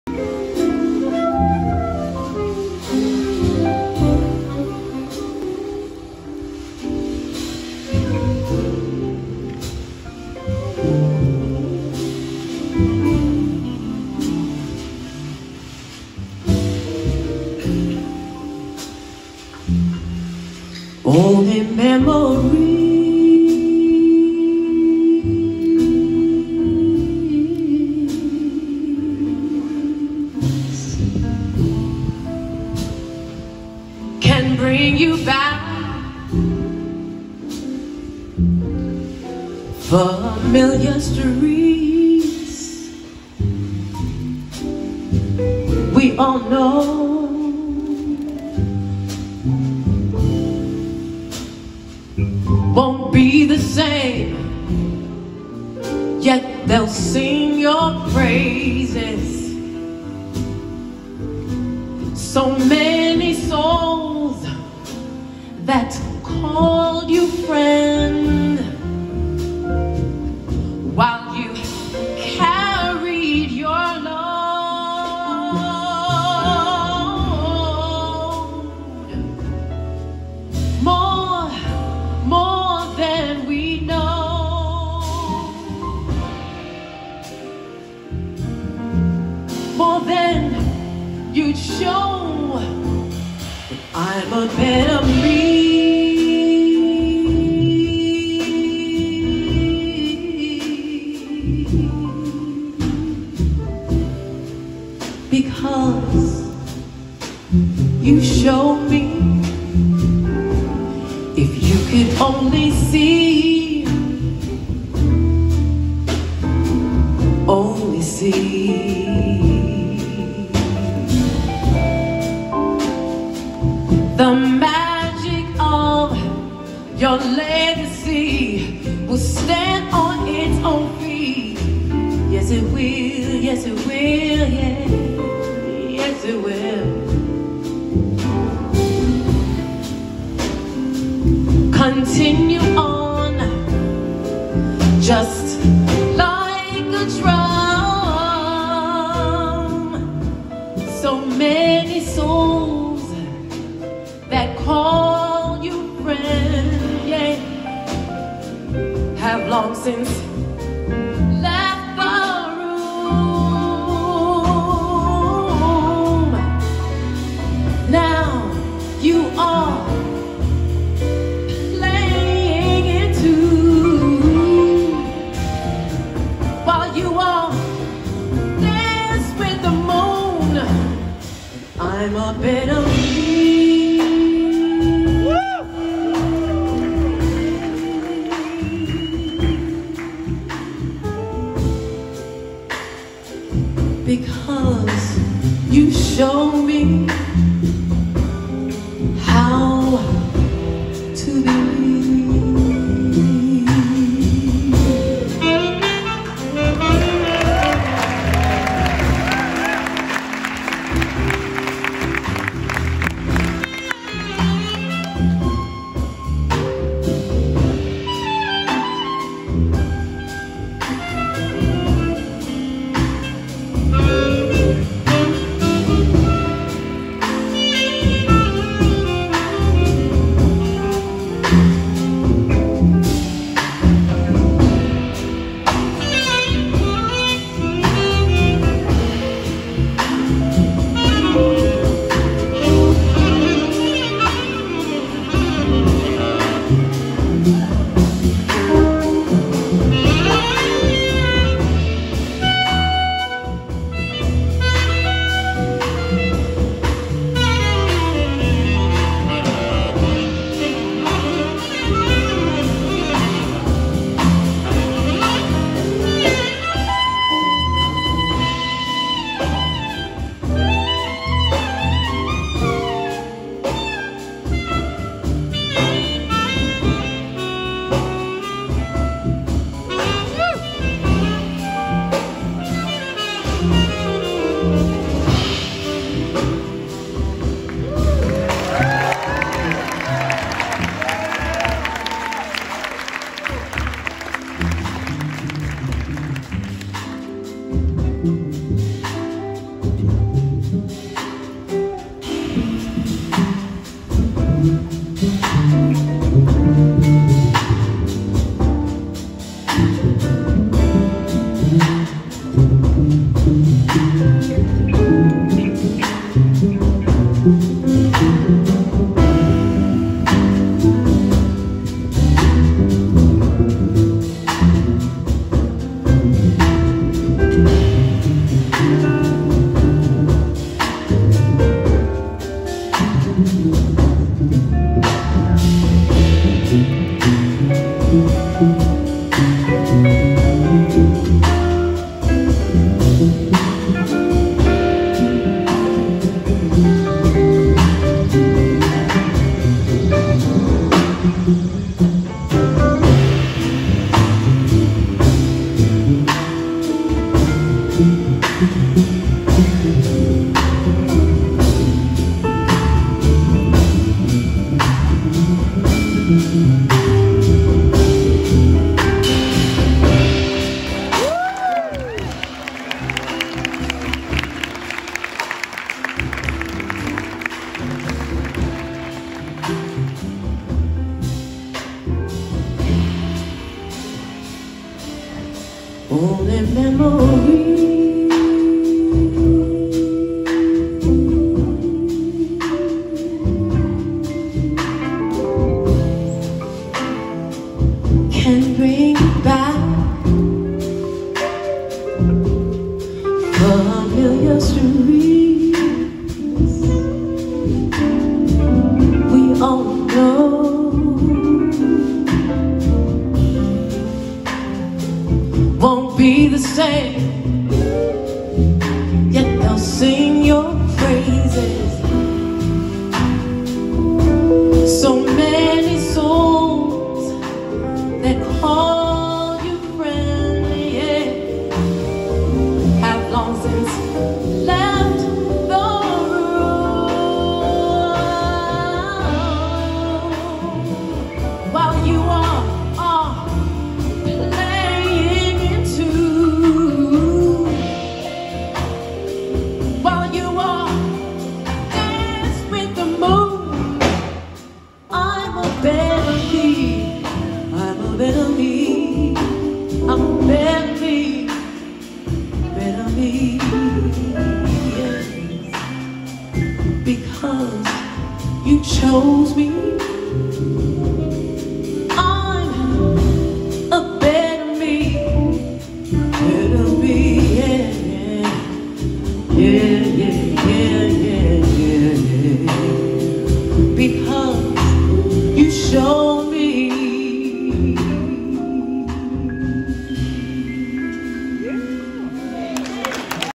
Jazz Gallery All-Stars 2021 feat. Renee Neufville
Ben Williams - bass
Marcus Gilmore - drums
Jaleel Shaw - sax
Mellissa Aldana - sax
Joel Ross - vibes
Gerald Clayton - piano
Charles Altura - guitar

Newport Jazz Fest 2021